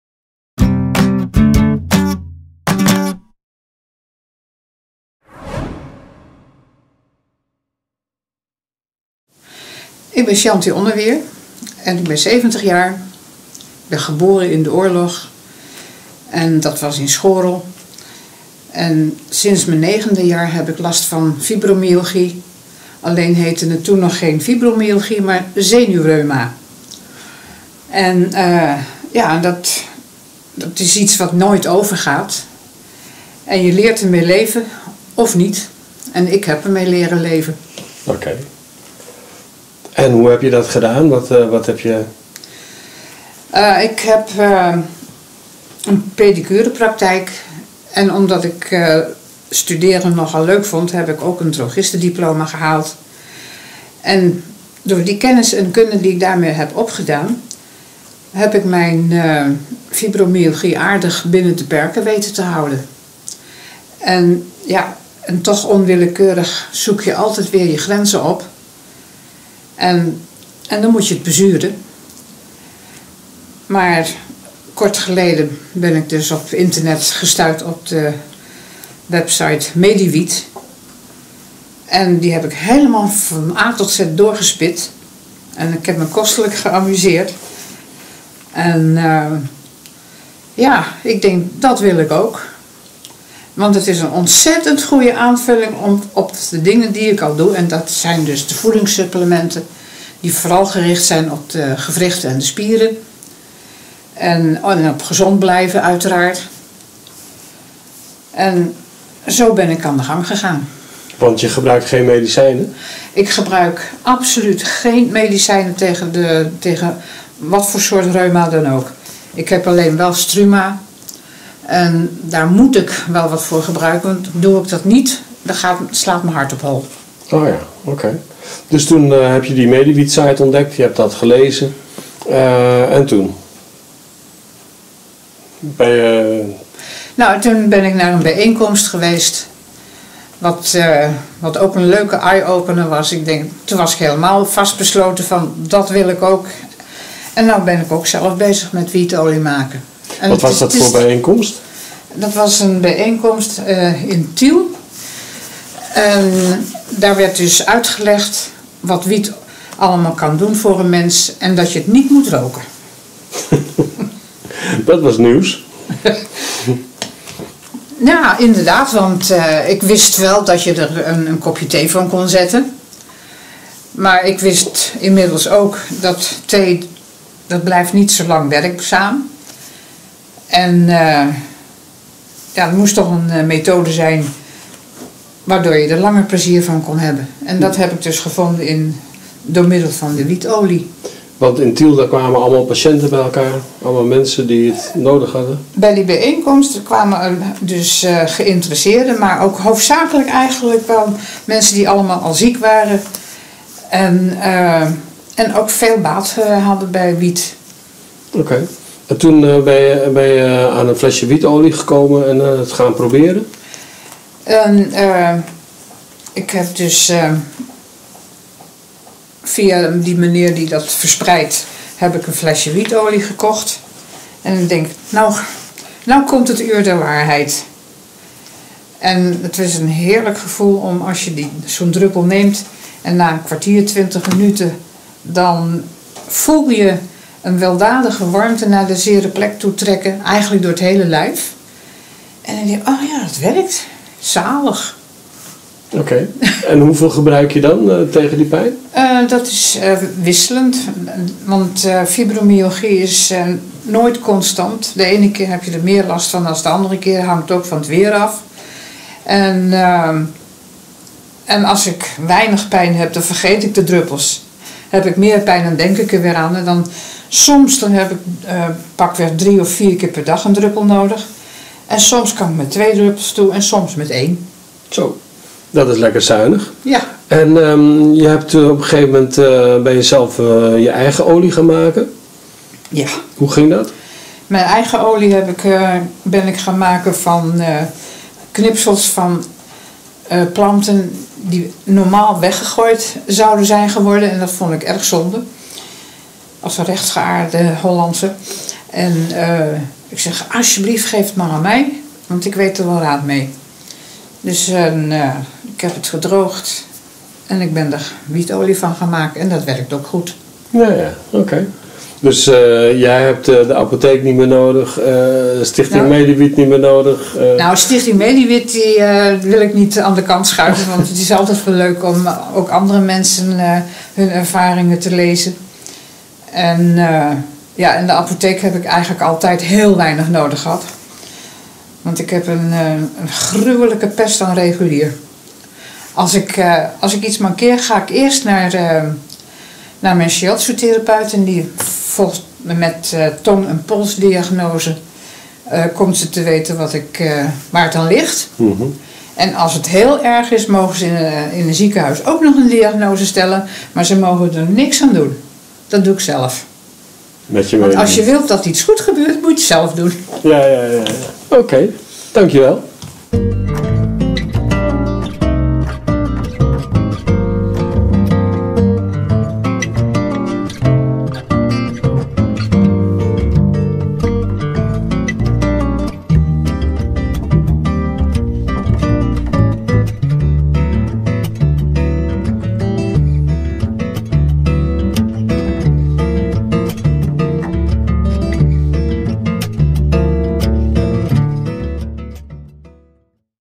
Shanti Onneweer maakt nu haar eigen olie, fybromyalgie
Patiënten interview met Shanti Onneweer.

Shanti lijdt al 61 jaar aan
fybromyalgie. Wietolie
heeft haar kunnen helpen
met slapen en om de
pijn dragelijk te maken,
waardoor zij nu normaler
door het leven kan.

Stichting Mediwiet